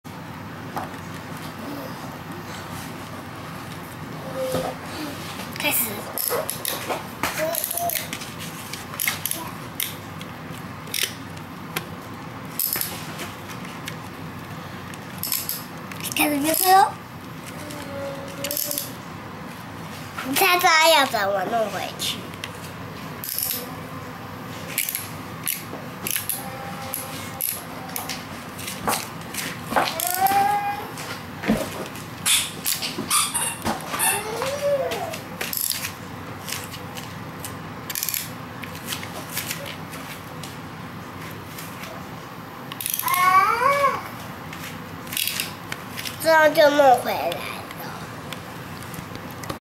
麵包超人魔術方塊_1
小朋友玩麵包超人魔術方塊